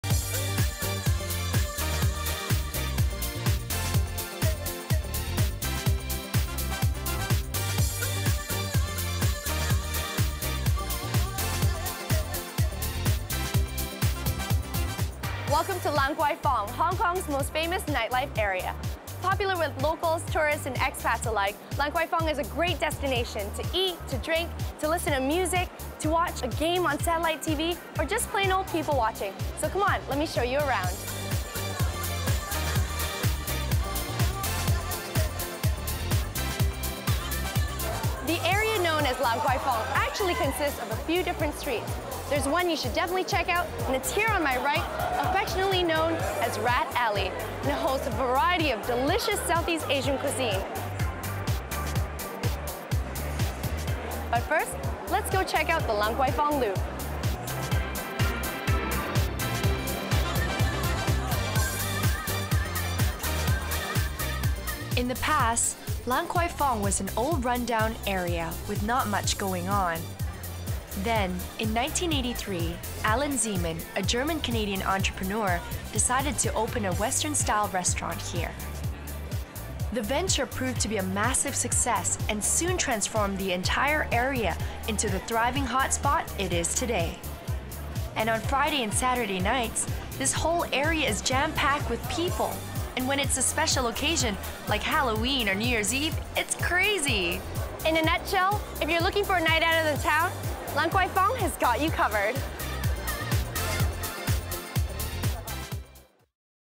Hong Kong - Lan Kwai Fong (LFK) Nightlife
Discover Hong Kong with us. Tonight we explore Lan Kwai Fong, the infamous nightlife and bar block in Hong Kong's Central district.